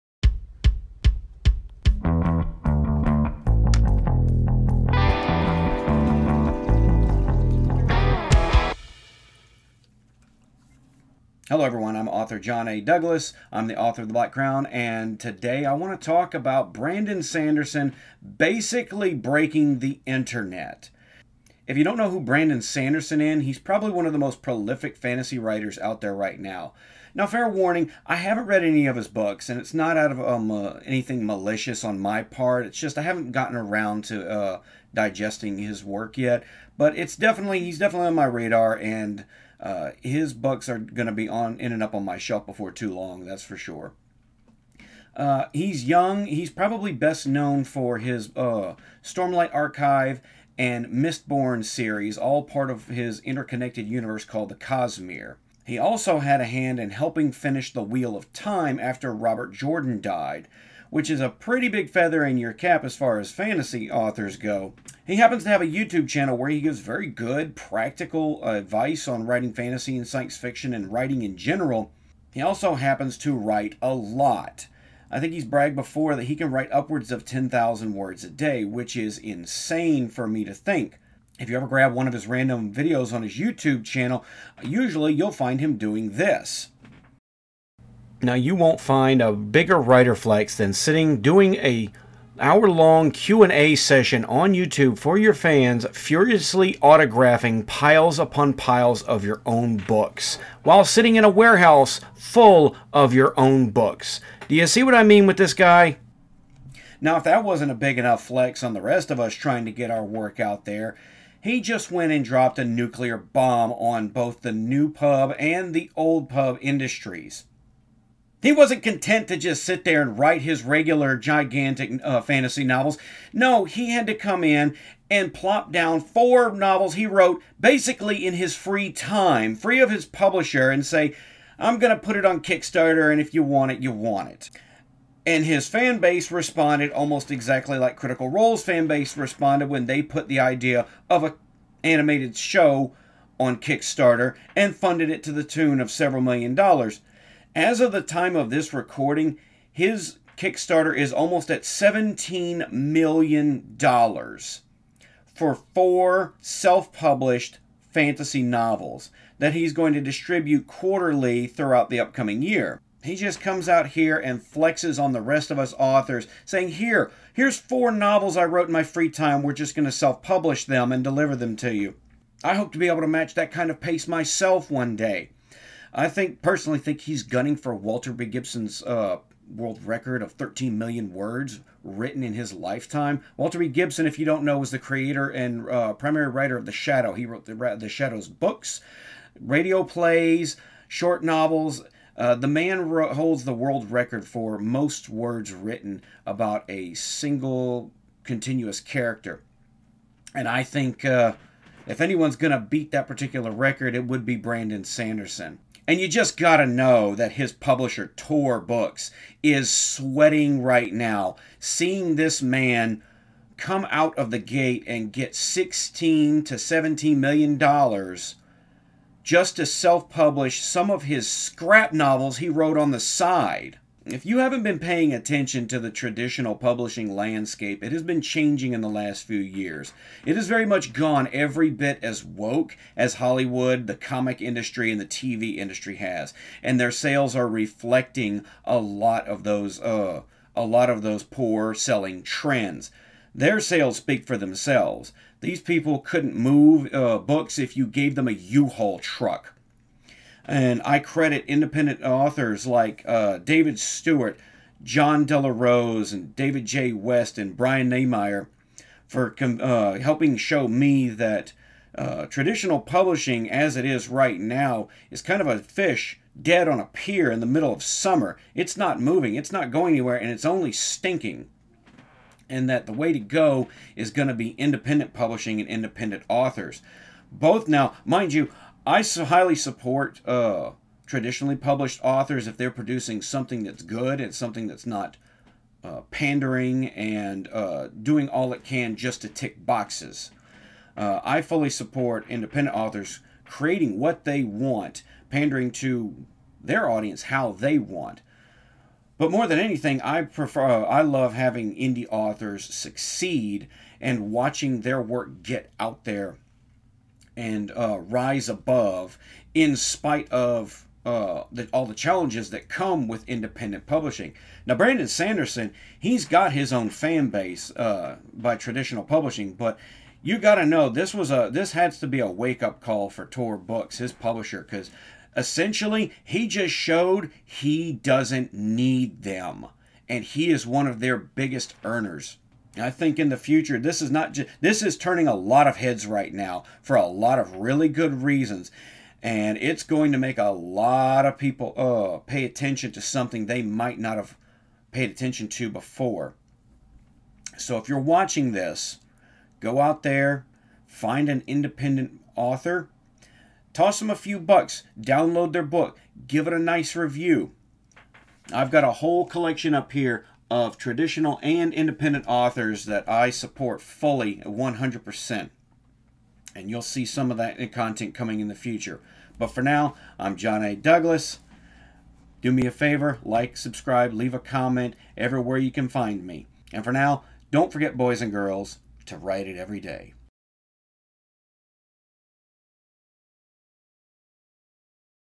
Brandon Sanderson Breaks The Internet
Brandon Sanderson decided to drop four unannounced novels he wrote in his free time and self-publish them. The internet reacted accordingly.

~Links~
Twitter: @J0hnADouglas  (With a zero!)